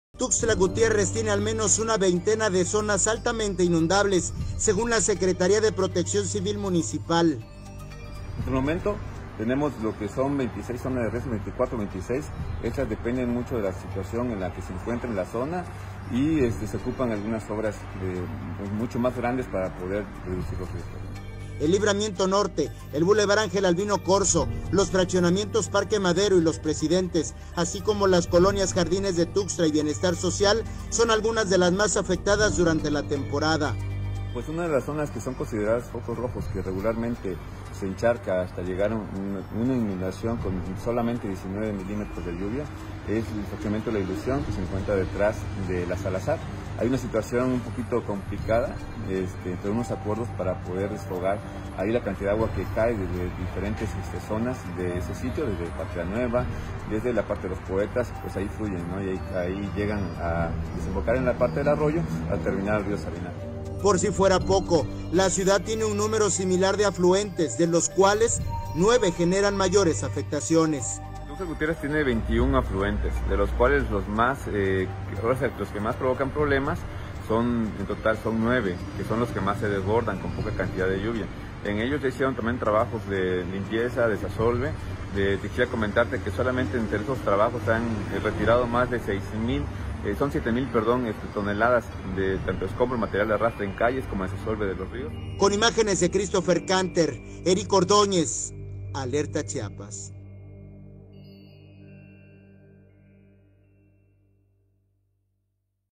Tuxtla tiene una veintena de zonas inundables
El Libramiento Norte, el Bulevar Ángel Albino Corzo; los fraccionamientos Parque Madero y Los Presidentes, así como las colonias Jardines de Tuxtla y Bienestar Social, son algunas de las más afectadas durante la temporada.